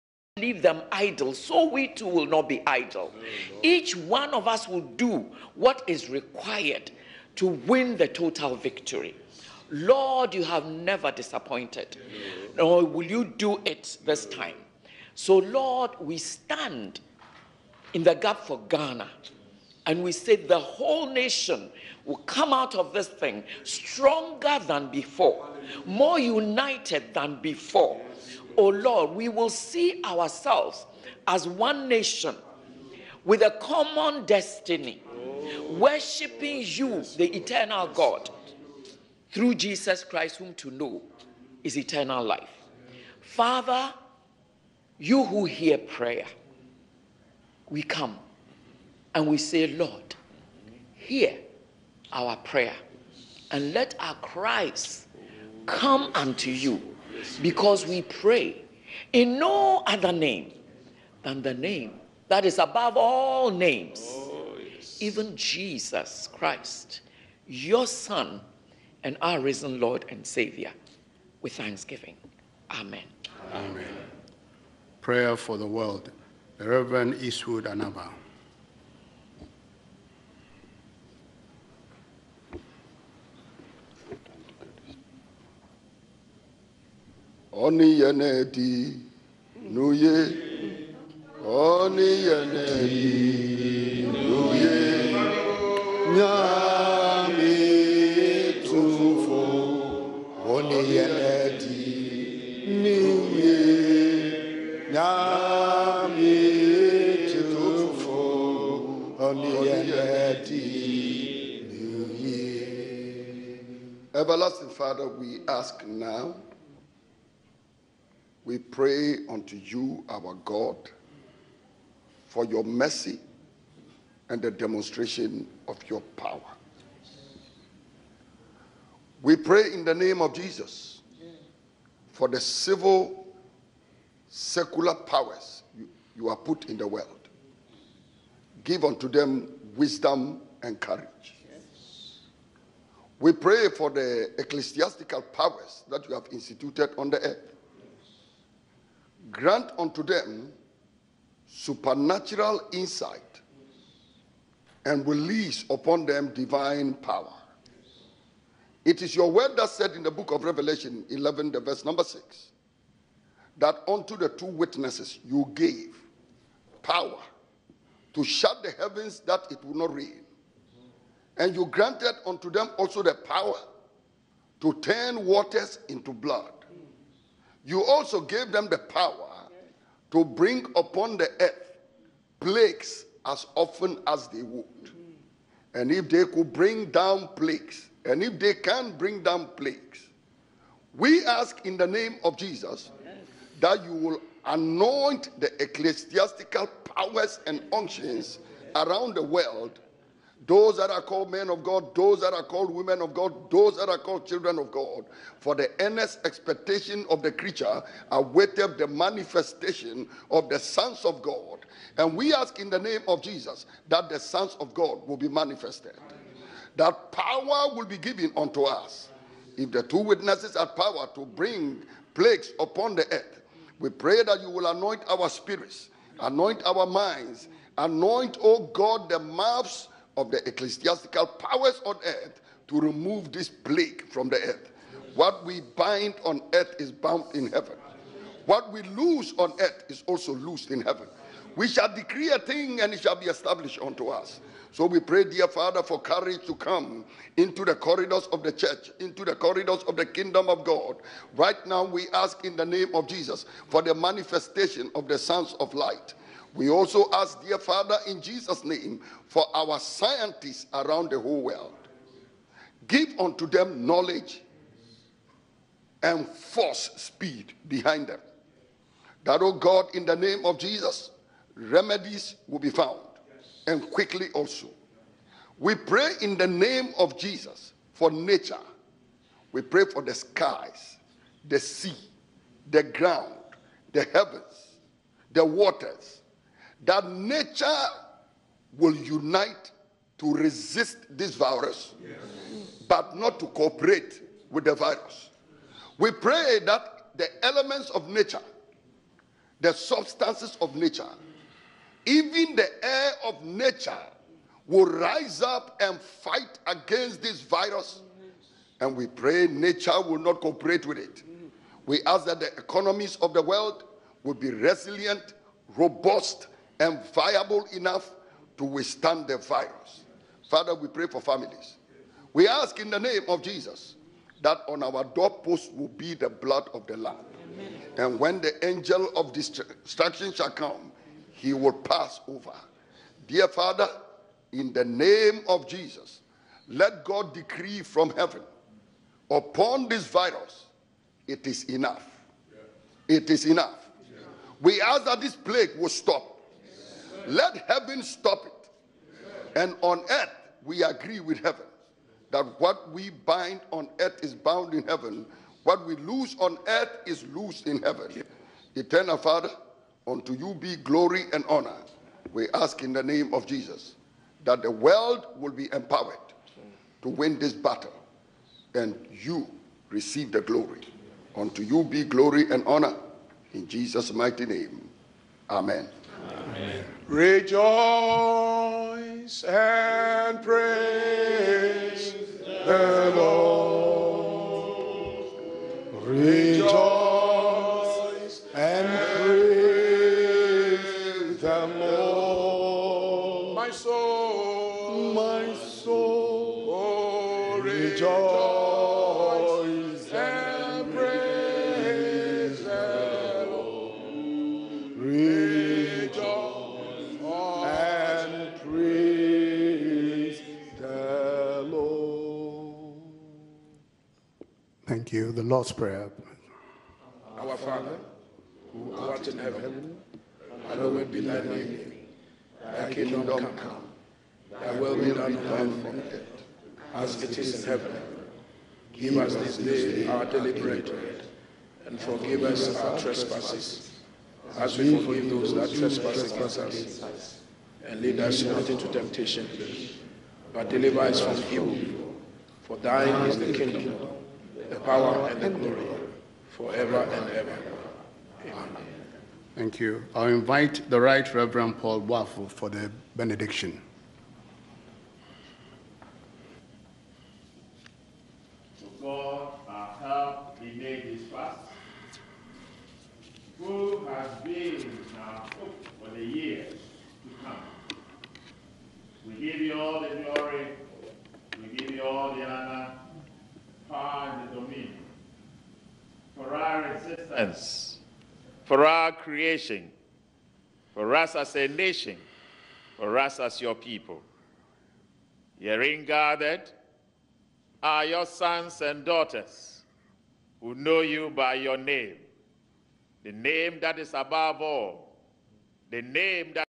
Fervent Prayers by Pastors in Ghana with the President against CONVID-19. God save the WORLD....
Prayers for the nations in the world against CONVID-19 pandemic. Emminent pastors in Ghana praying for nations to by heal by the power of God.